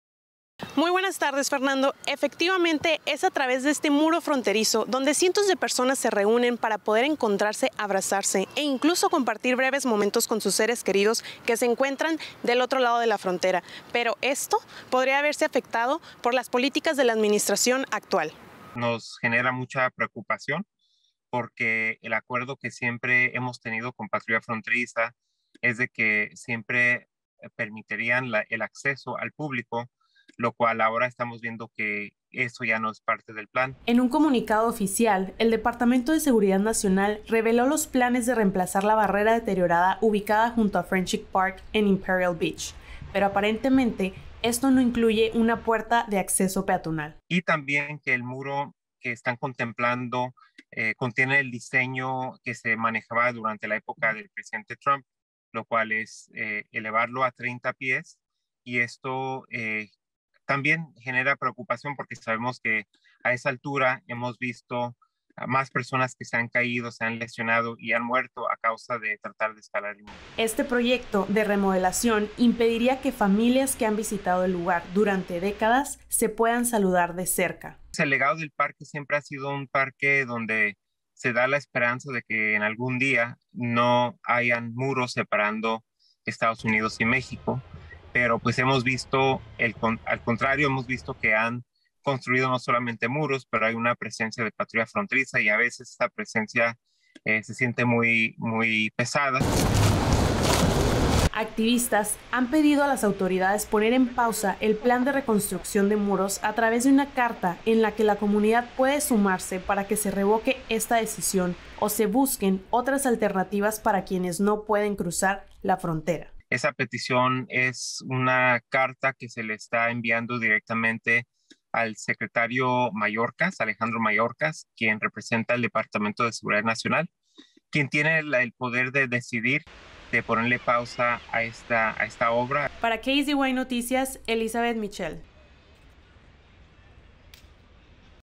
Remodelación de muro fronterizo impedirá que familias se reúnan en Friendship Park.
Durante décadas las personas que no pueden cruzar la frontera tienen la oportunidad de encontrarse con sus seres queridos en Friendship Park, pero esto podría terminar por la una remodelación del muro fronterizo.